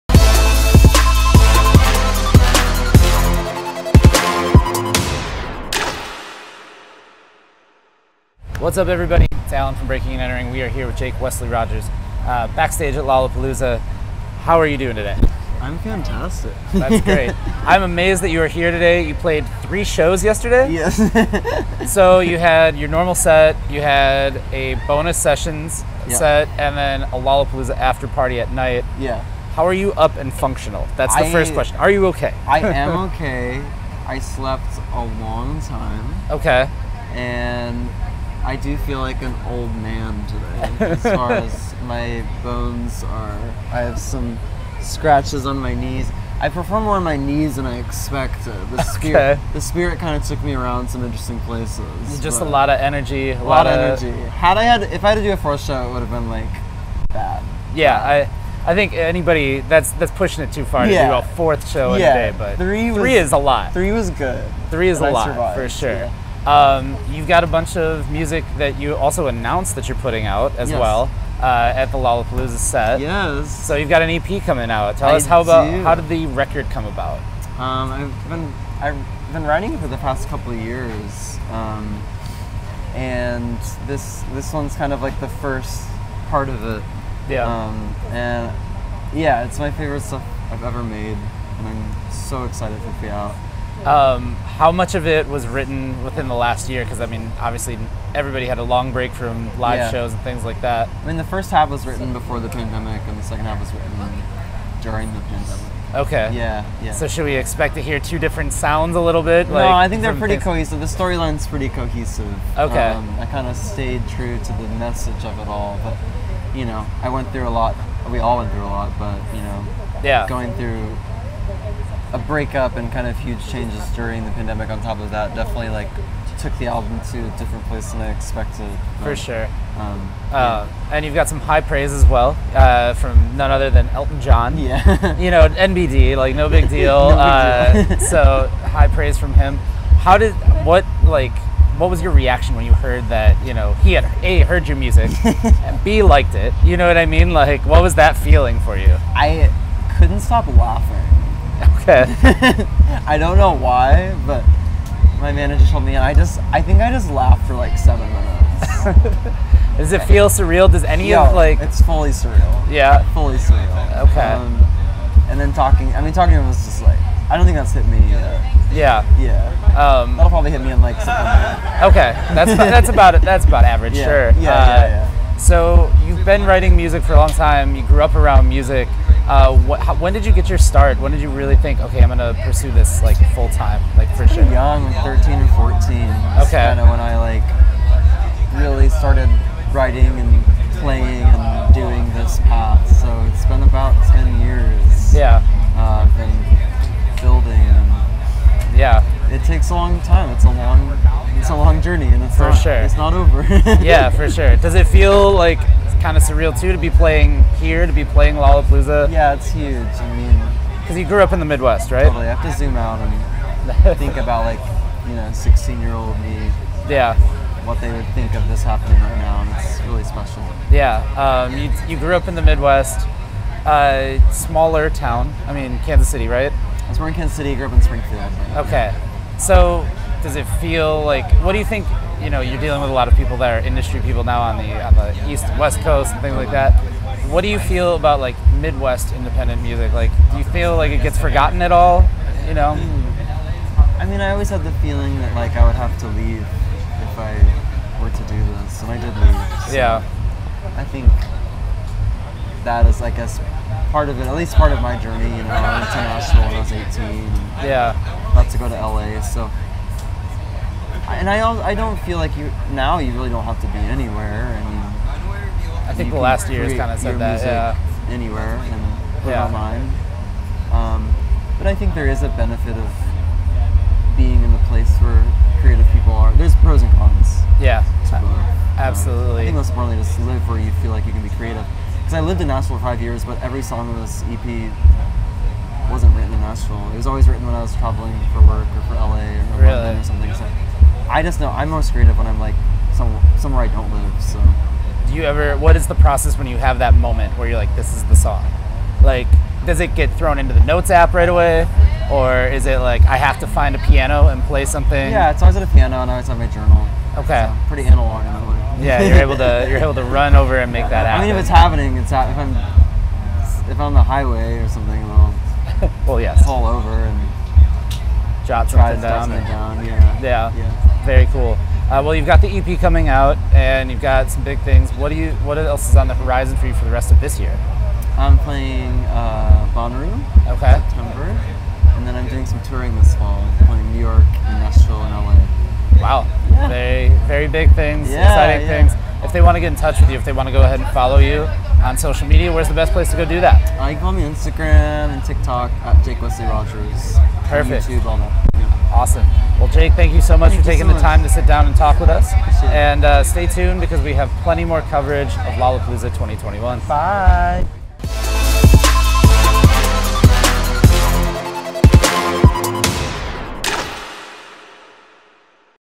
INTERVIEW: Jake Wesley Rogers
We caught up with Jake Wesley Rogers at Lollapalooza 2021 to discuss the three shows that he played in one day, getting praise from Elton John and much more.

Get the latest in Milwaukee music daily at BreakingAndEntering.net

Facebook.com/BreakingAndEntering1
Instagram.com/BreakingAndEntering1
Twitter.com/BreakinNEnterin
Snapchat.com/add/BreakinNEnterin